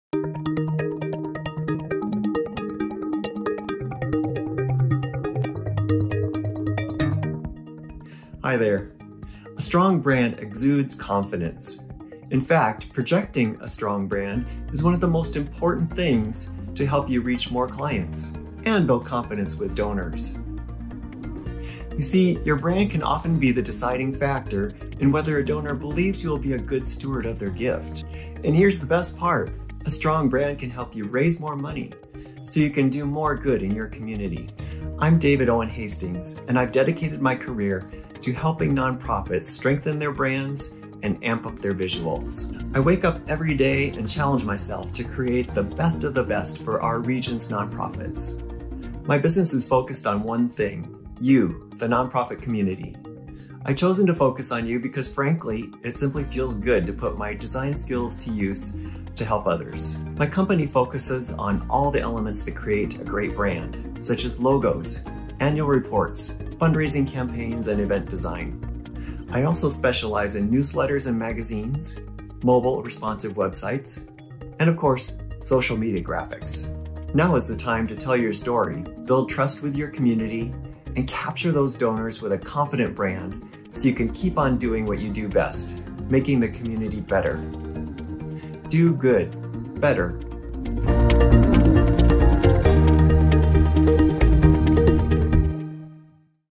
David Owen Hastings Design 2020
I'm David Owen Hastings. In my graphic design office, I work exclusively with nonprofit organizations, helping them connect with donors, supporters, clients and the community through distinctive branding and marketing design tools. I provide a high level of experience and expertise at rates my nonprofit clients can afford.